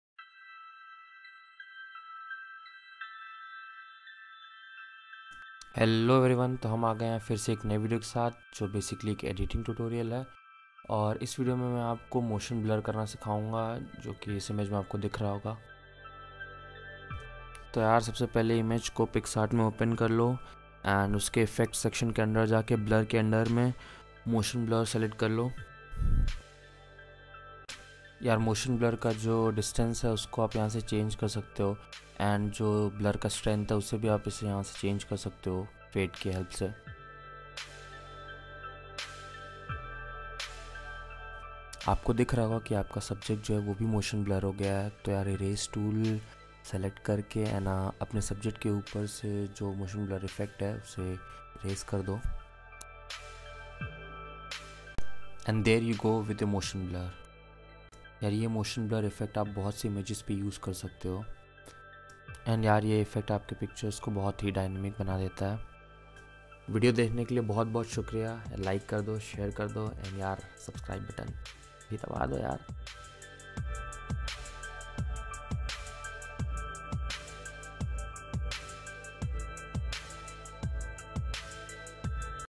How to add Motion Blur in your photographs | Picsart Editing Tutorial
In this video i have given a editing Tutorial on how to add Motion Blur in your Photographs using Picsart.

Comment and tell me if you want more Editing Tutorials.

In this video i am sharing an amazing editing tutorial to Add Motion Blur Effect to your photographs.